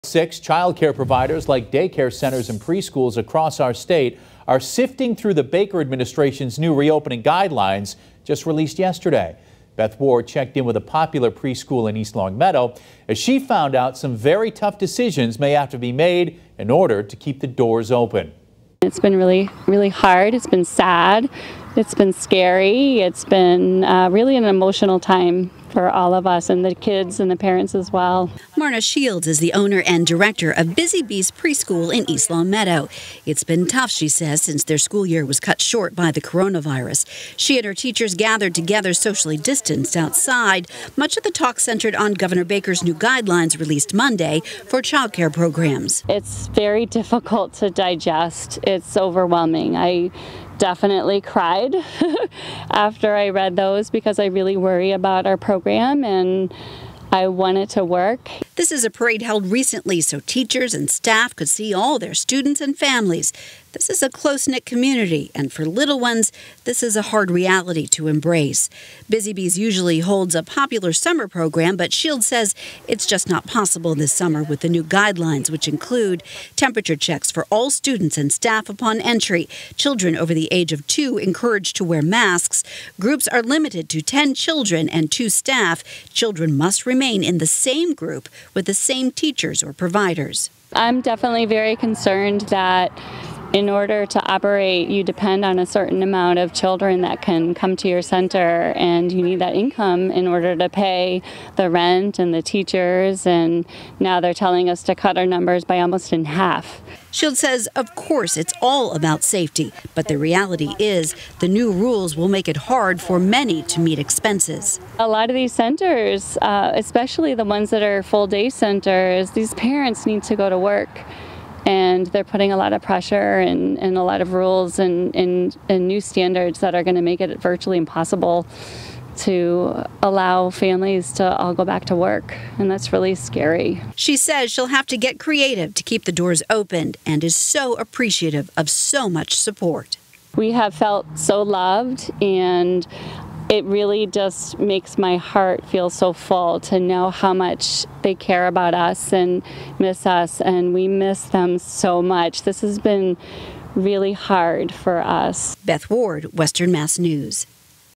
East Longmeadow preschool to close summer program
Some very tough decisions may have to be made to keep the doors open.